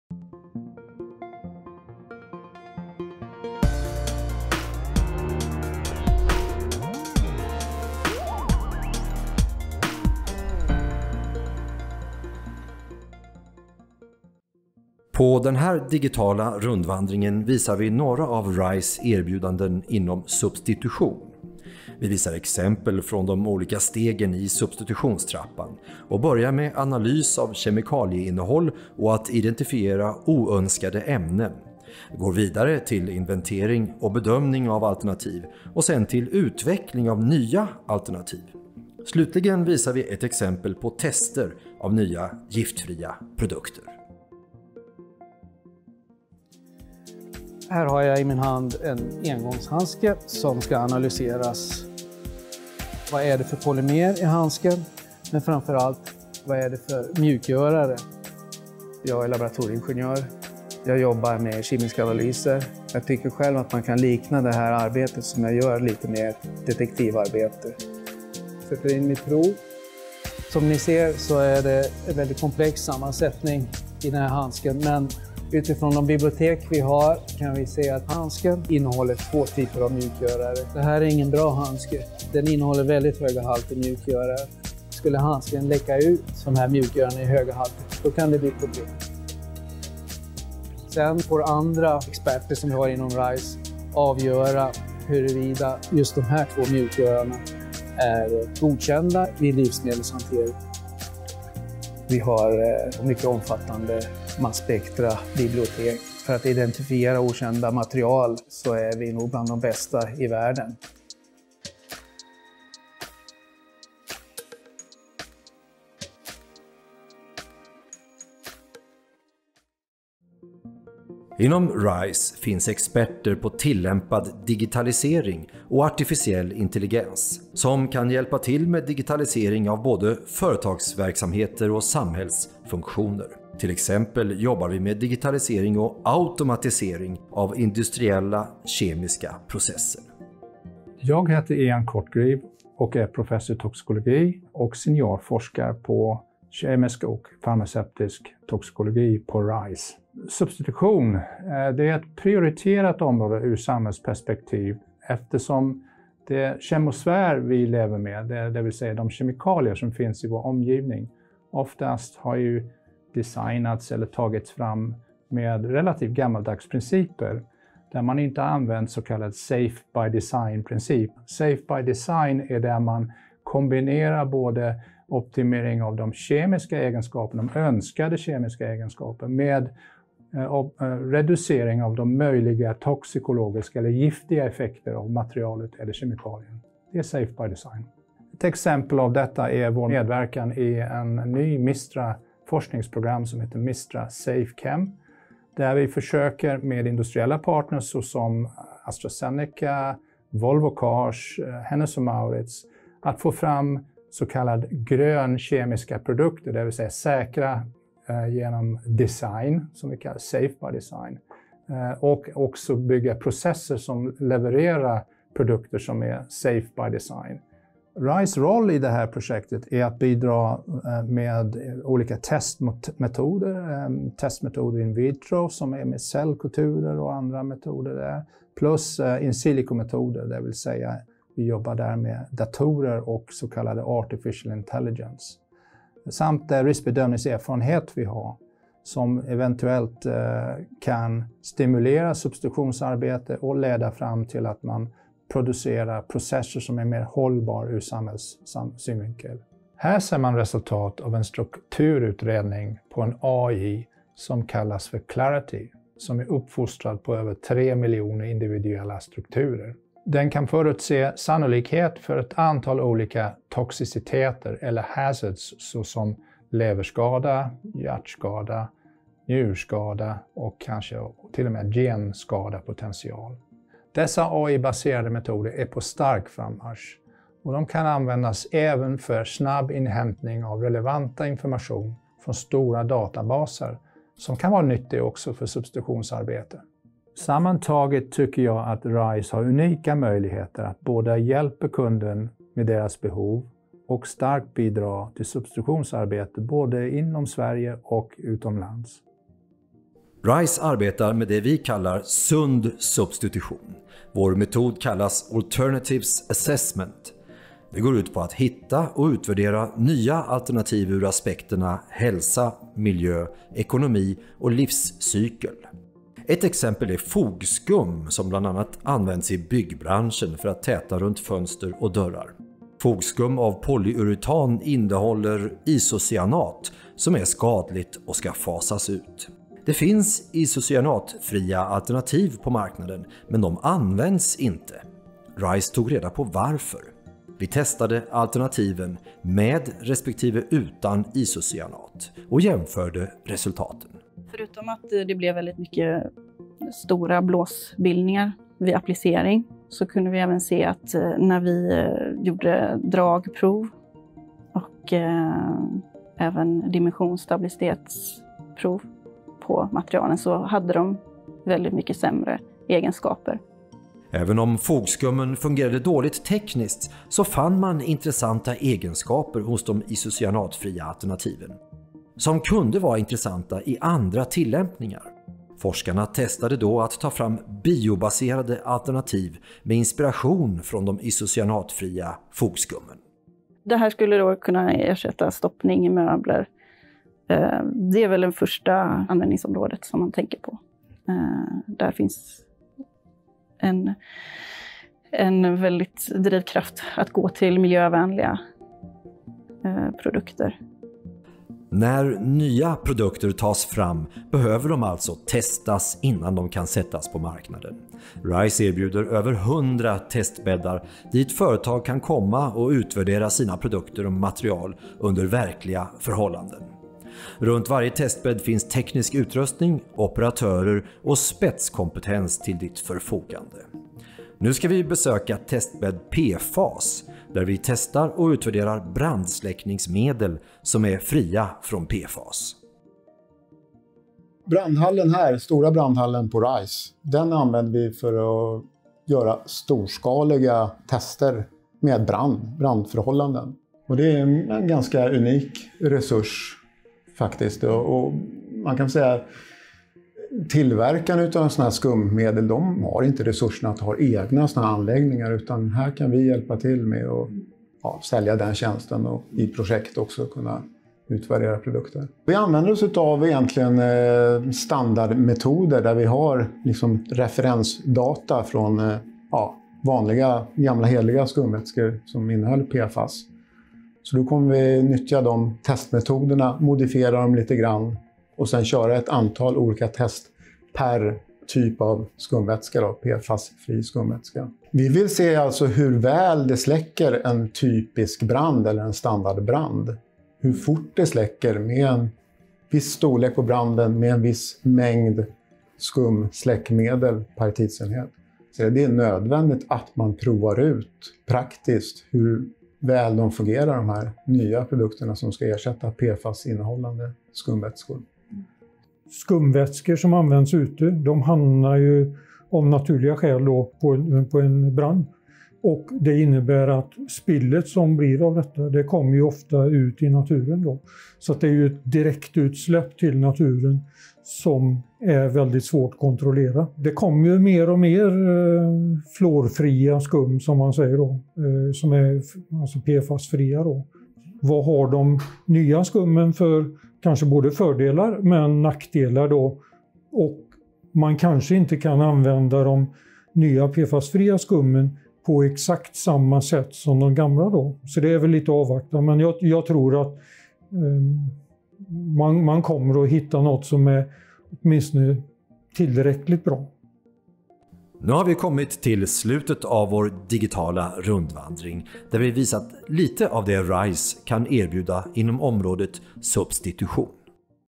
What RISE can offer in the field of substitution
On this digital tour, we will show you some of what RISE can offer in the field of substitution. You will see examples from the different stages of the substitution ladder.